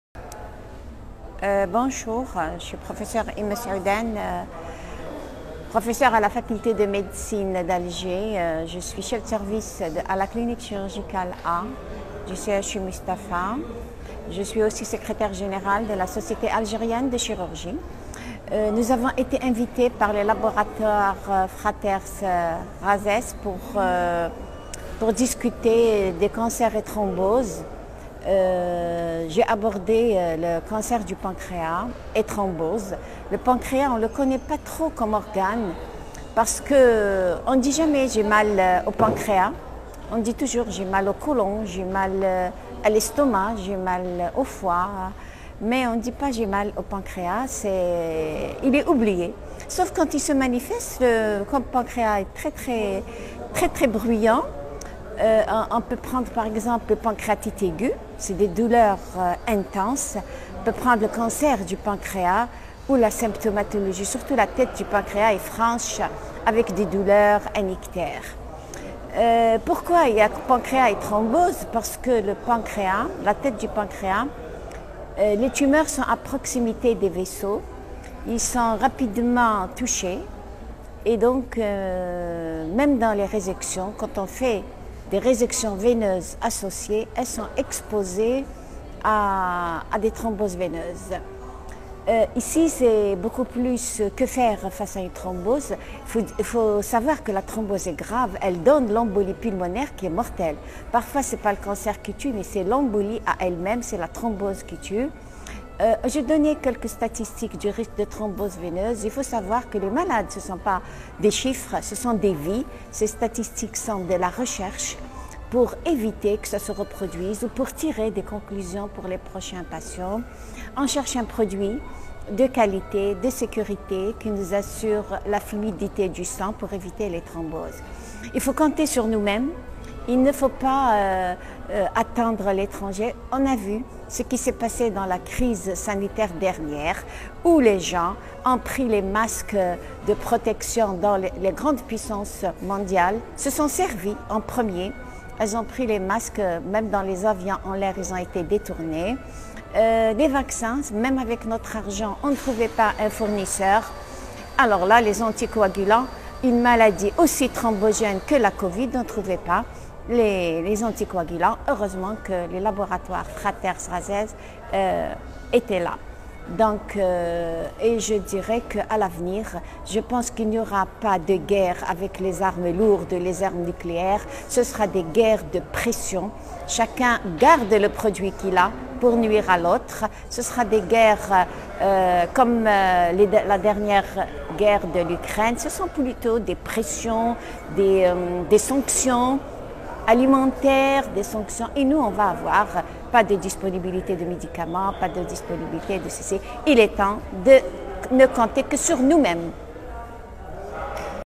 Pr Zohra Imessaoudene- Varenox chirurgie carcinologique digestive
Thrombose veineuse et cancer du pancréas ; une association fréquente et redoutable est l’intervention du Pr Zohra Imessaoudene, cheffe de service de la clinique Chirurgicale A au CHU Mustapha en marge du symposium sur le varenox en chirurgie carcinologique digestive.